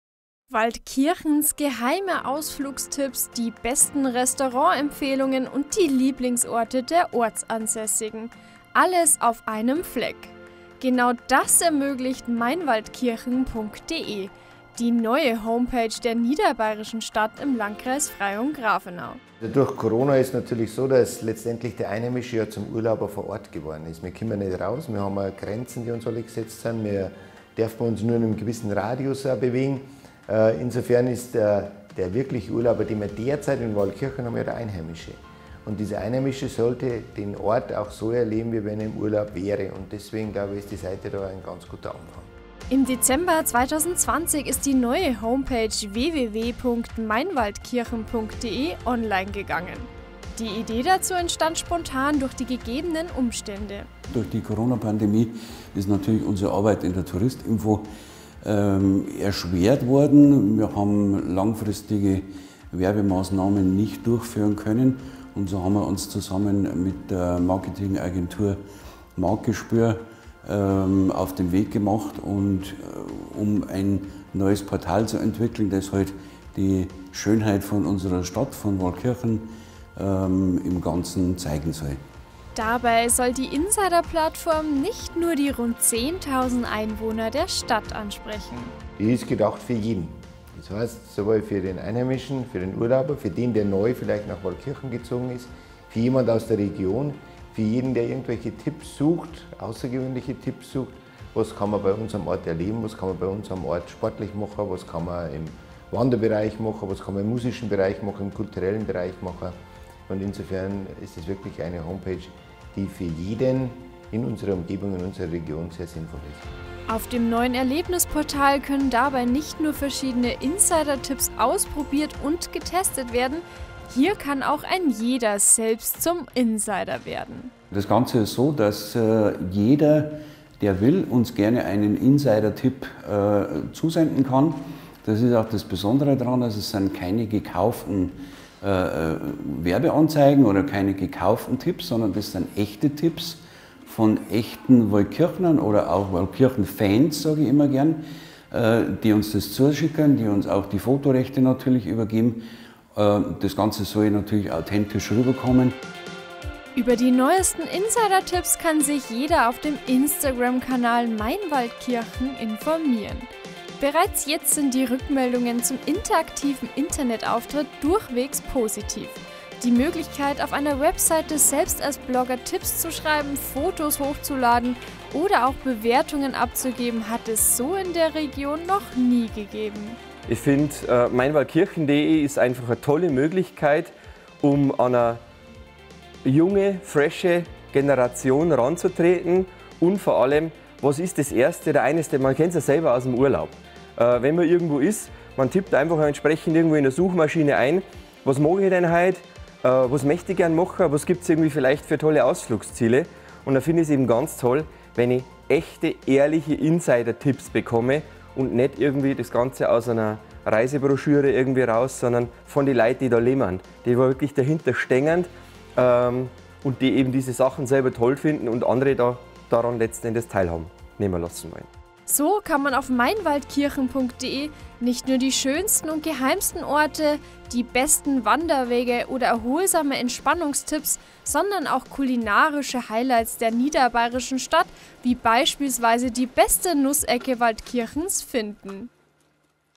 Mein Waldkirchen - das neue Erlebnisportal
MeinWaldkirchen.de ist mehr als nur eine einfach Homepage. Es ist ein Erlebnisportal einer wundervollen Kleinstadt im Bayerischen Wald, gespickt mit Stories von echten Waldkirchnern, mit Insider-Tipps rund um Waldkirchen und vieles mehr. Natürlich findet man auf dem Portal alles was man in und um Waldkirchen erleben kann und für den Urlaub in Waldkirchen gibt's den passenden Gastgeber obendrauf auf dem Portal zu finden, logischerweise gleich mit Kontaktdaten!